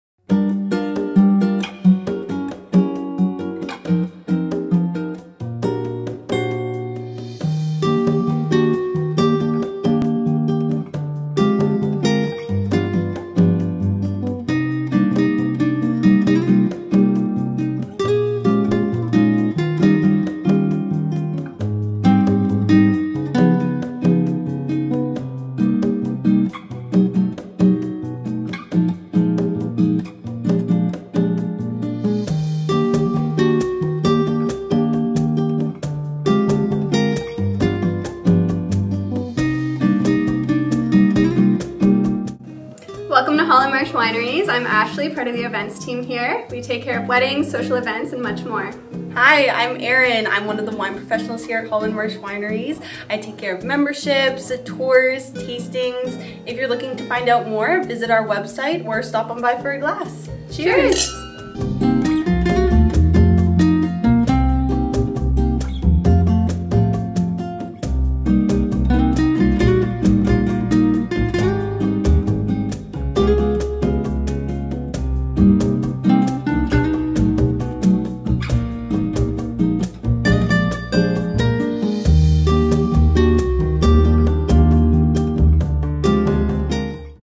Holland Marsh Wineries
Welcome to Holland Marsh Wineries, our May “Eat, Drink & Play in the Neighbourhood” feature🍷

Located at 18270 Keele Street in Newmarket (nestled in The Holland Marsh), the Nersisyan family purchased the @hollandmarshwineries 22-acre property and planted the first block of grape vines in 2007 - opening it’s doors for business in 2010! 🍇